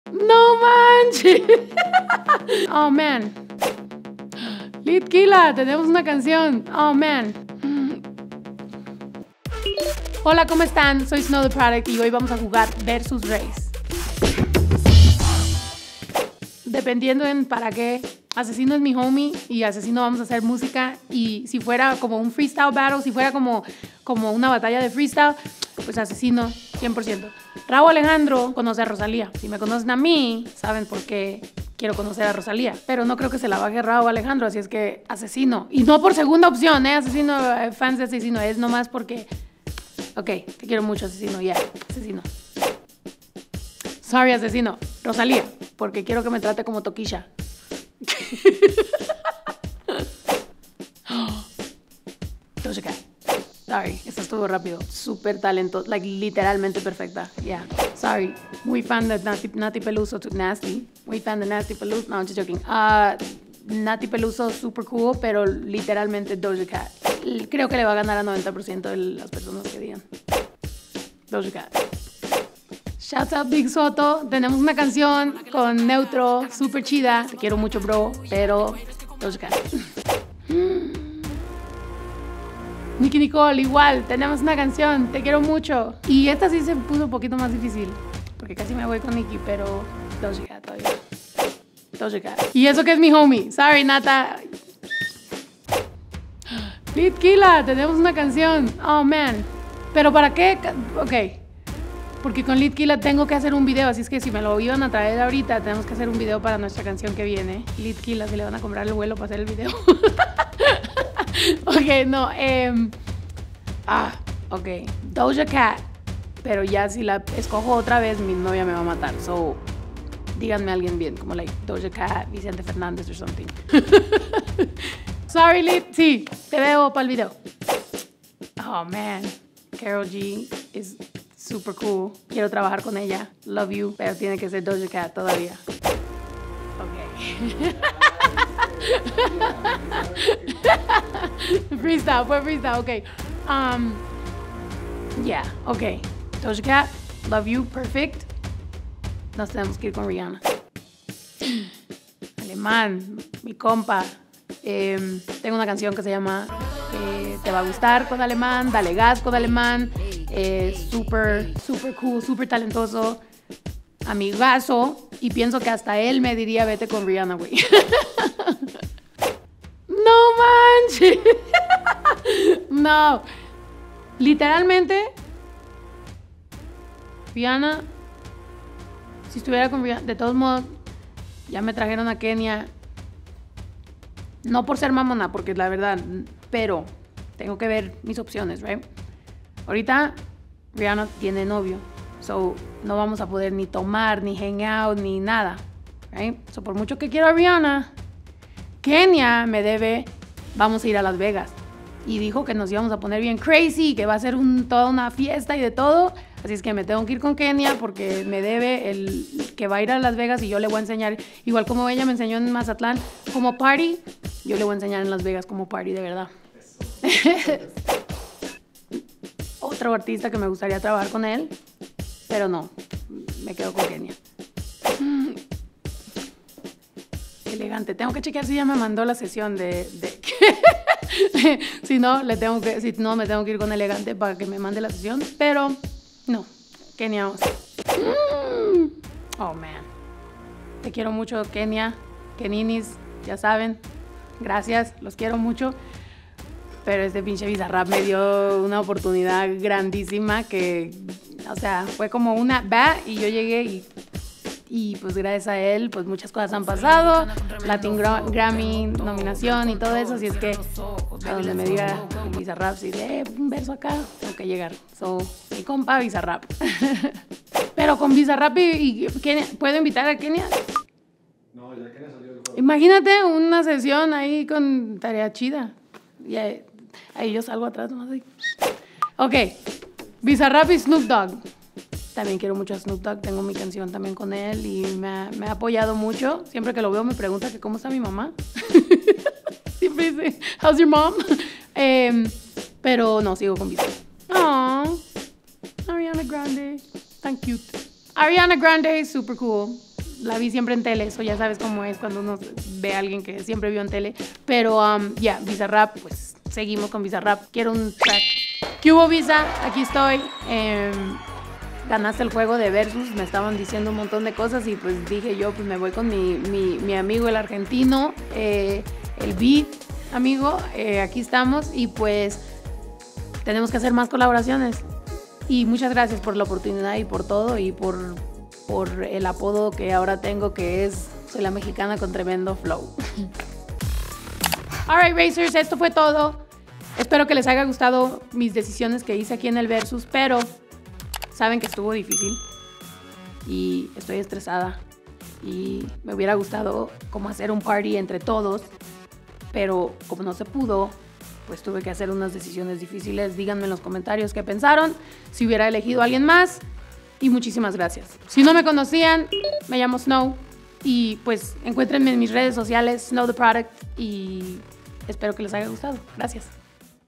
¿Snow Tha Product se va con Kenia Os de party? ¿Rosalía Doja Cat? | Versus Raze
¿Snow Tha Product entra en crisis? ¿Rauw Alejandro o Aczino? ¿Rosalía o Doja Cat? ¿Nicki Nicole o Bizarrap? ¿Karol G o Rihanna?😱 No vamos a dar spoilers, ¡tendrás que ver hasta el final de este video para enterarte!❤️‍🔥